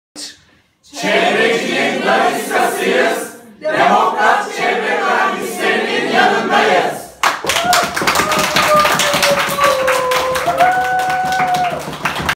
Kuzey Ormanları Savunması: Çevrecinin daniskasıyız, "Demokrat Çevre Mühendisleri"nin yanındayız.
31 Ocak 2016 günü yapılacak Çevre Mühendisleri Odası İstanbul Şubesi seçimlerinde "Demokrat Çevre Mühendisleri"nin yanındayız...